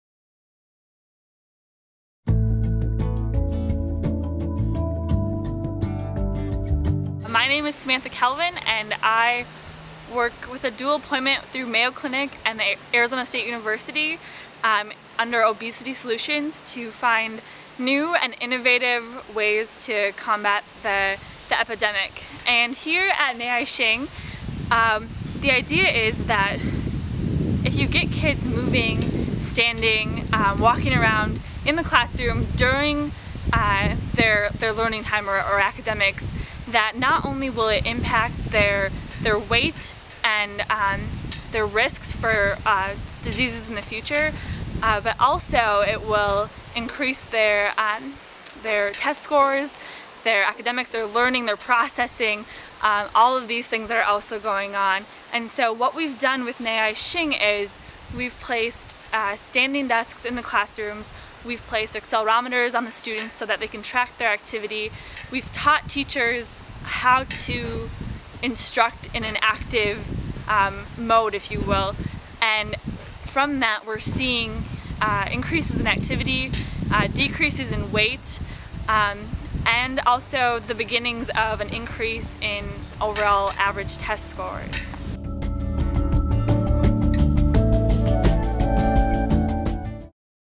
Ergotron WorkFit®: Sit-Stand Desks in the Classroom
Transform classrooms into active learning environments! A researcher describes the health benefits of standing desks at Nay Ah Shing school. Further data also suggests test scores being positively impacted.